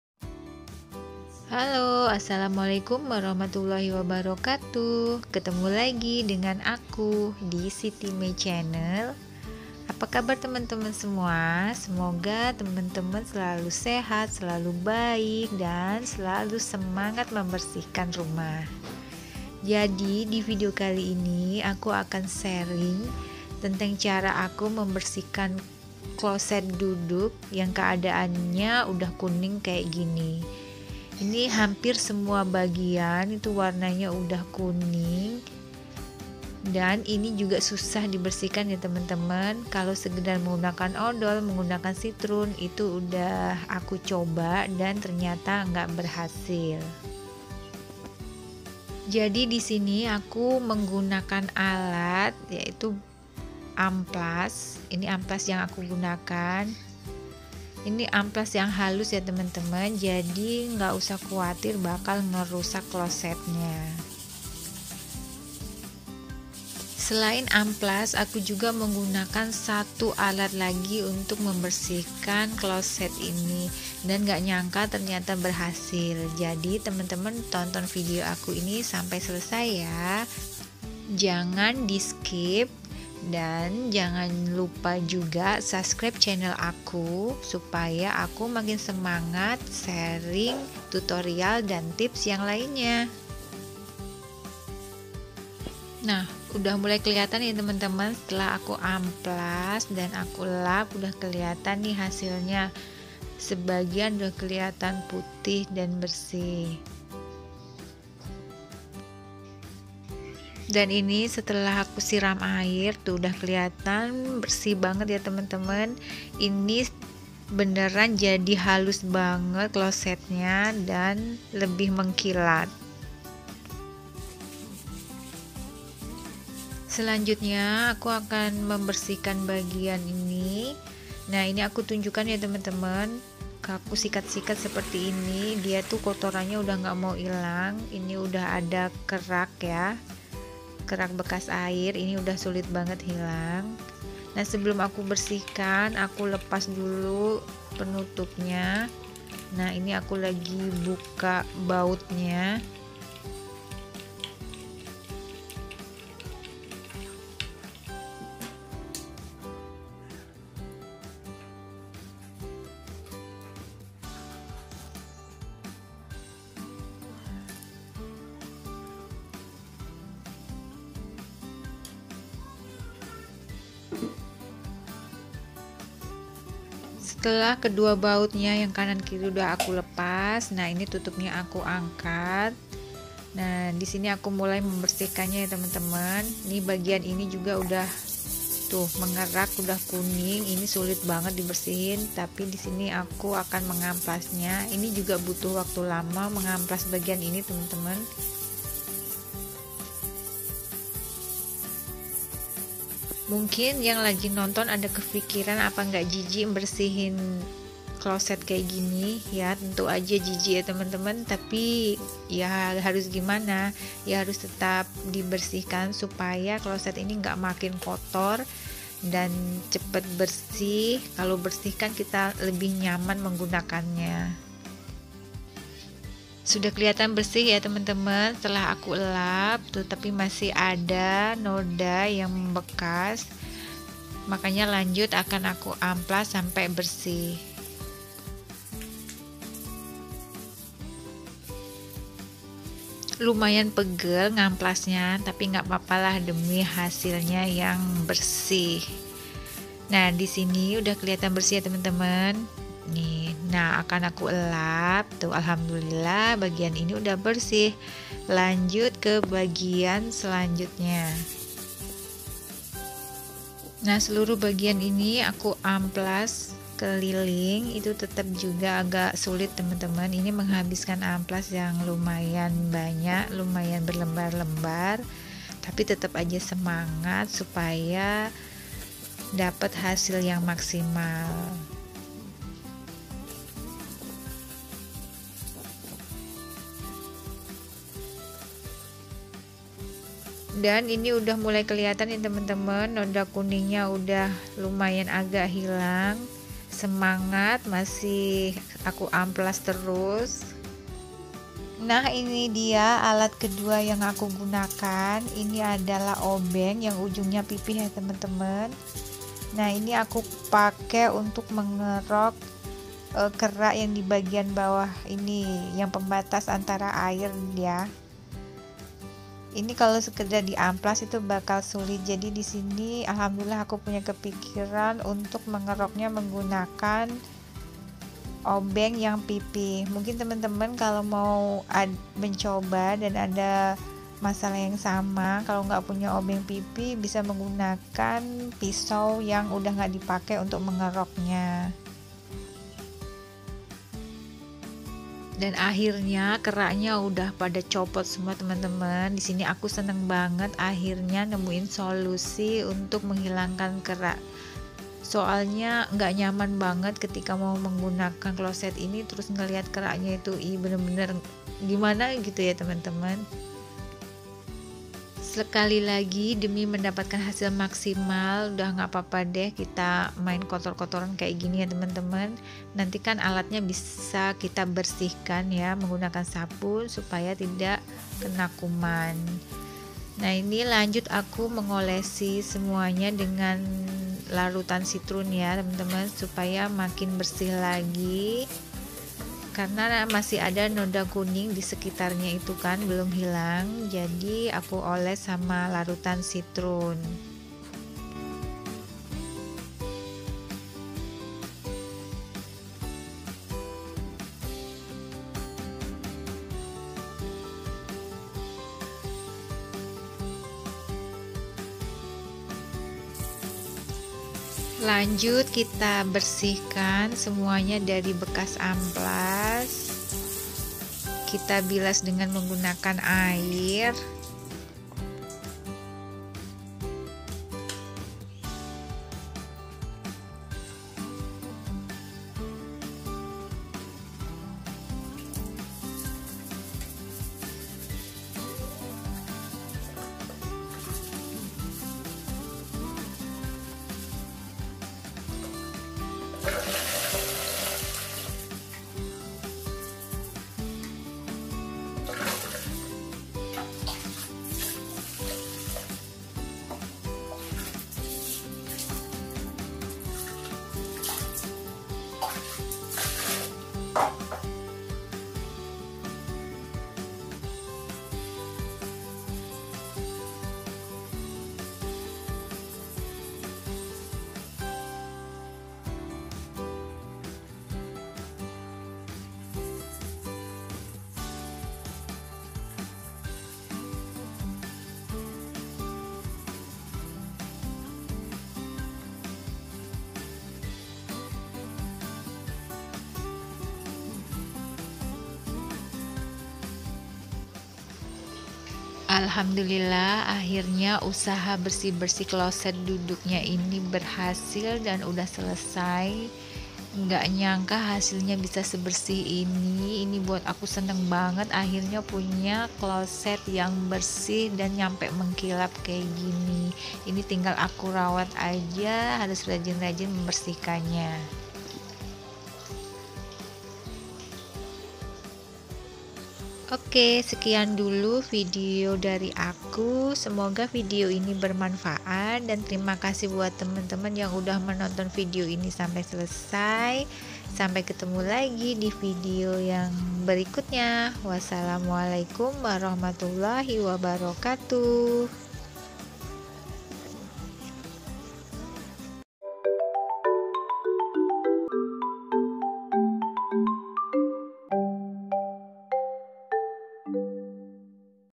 CARA AMPUH MEMBERSIHKAN CLOSET DARI NODA BERKERAK | kloset jongkok / duduk juga bisa dengan cara ini
assalamualaikum,hai semua nya,,
di video ini aku sharing tentang cara membersihkan noda kuning dan kerak yg ada pada closet duduk,,
semoga video aku bermanfaat dan buat temen2 yang punya masalah yang sama pada closet nya bisa di cobain cara ini ya,,

dan jangan lupa aktifkan lonceng nya supaya gak ketinggalan video terbaru aku 🤗

trimakasih,,,